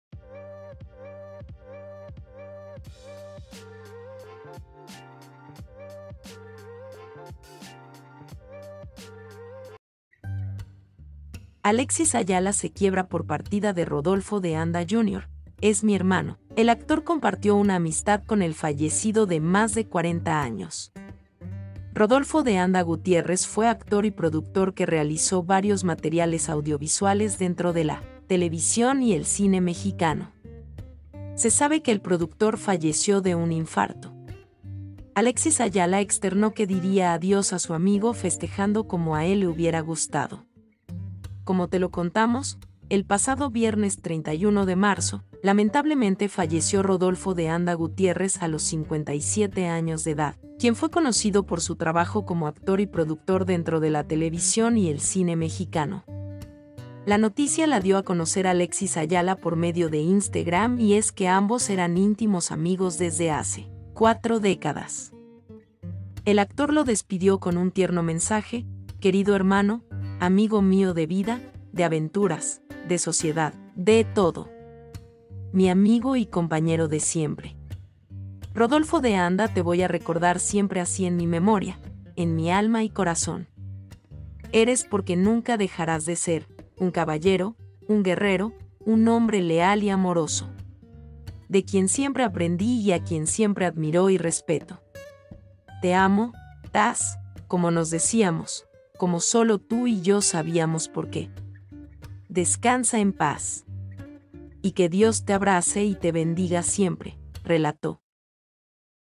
Alexis Ayala se quiebra por partida de Rodolfo de Anda Jr Es mi hermano
Este Contenido Es De Nuestra Propiedad.

Contenidos Creados Por : @conexiondigital.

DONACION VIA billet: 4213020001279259